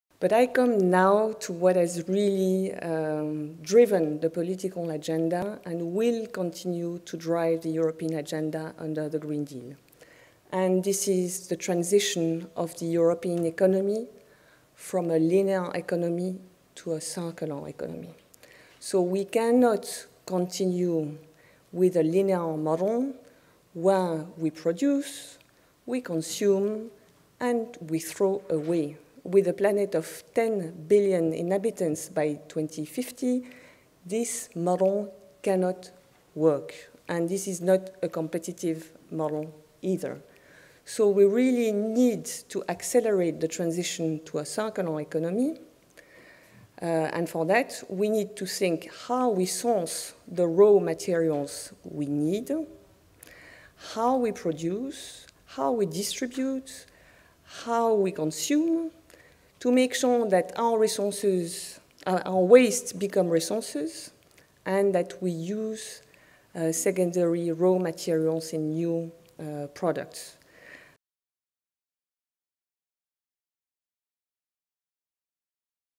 Emmanuelle Maire, Head of Unit, DG Environment, European Commission speaks at CECESummit 2019
Emmanuelle Maire, Head of Unit “Sustainable Production, Products & Consumption”, DG Environment, European Commission explains the importance of accelerating the transition towards circulareconomy in Europe during the Plenary Session at CECESummit